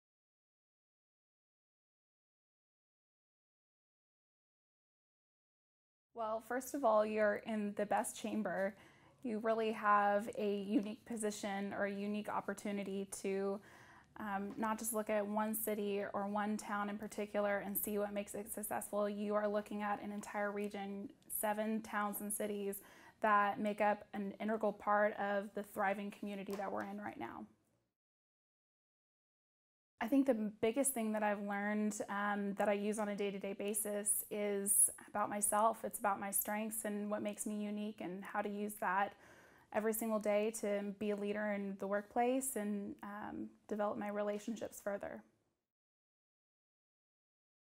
Regional Advantage Testimonial: Allison Hayes, Frost Bank
Allison Hayes with Frost Bank shares her experience with the Northwest Metroport Chamber's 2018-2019 Regional Advantage Leadership Class.